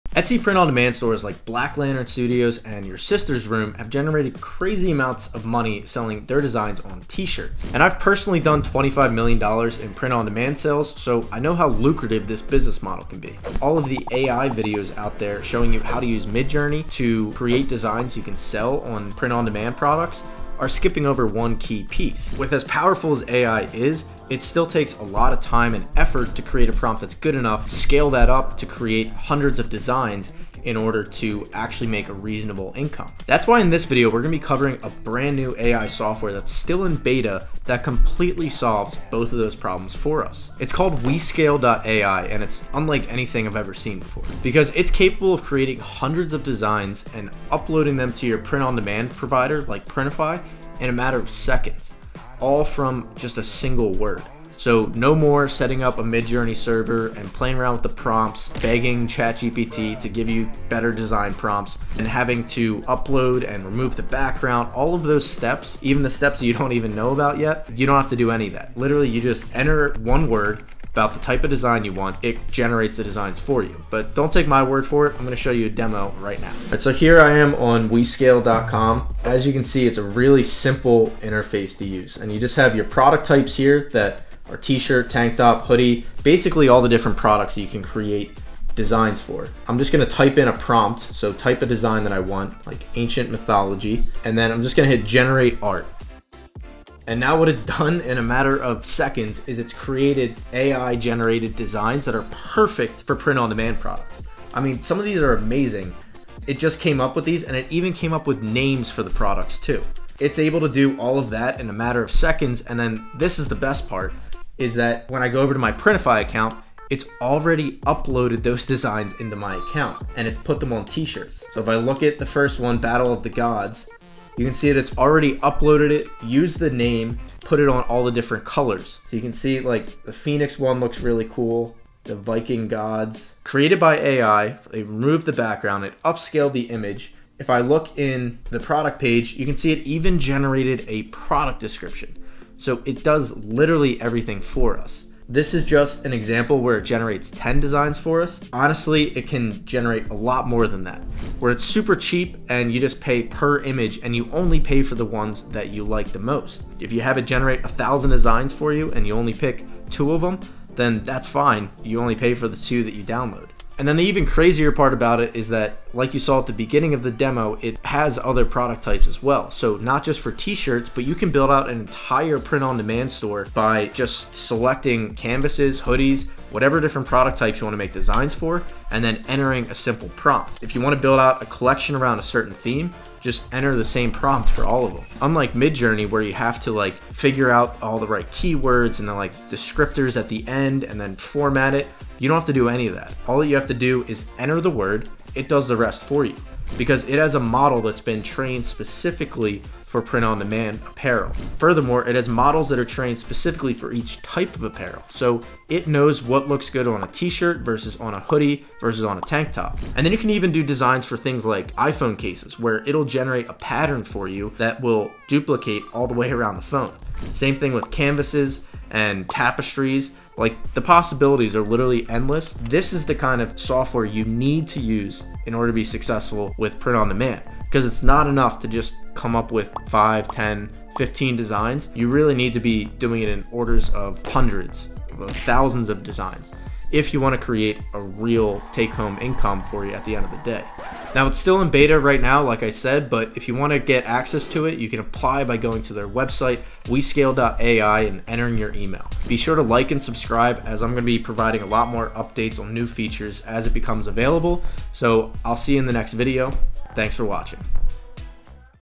How To Create INFINITE Midjourney Designs in Seconds (Easy Hack)
It's no news that many e-commerce business are now leveraging AI. But as powerful A.I is, it still takes a lot of time and effort to create a prompt that's good enough to scale up and create infinite designs. This video covers a tutorial on a brand new A.I tool called wescale.ai that makes a great hack to solve these problems.

► VIDEO TIMESTAMPS:
    0:00 Intro
    0:33 Brand New AI Tool
    1:18 AI Demo / AI Walkthrough
    2:32 AI Overview
    4:19 Designed for Print-on-Demand
    4:01 Outro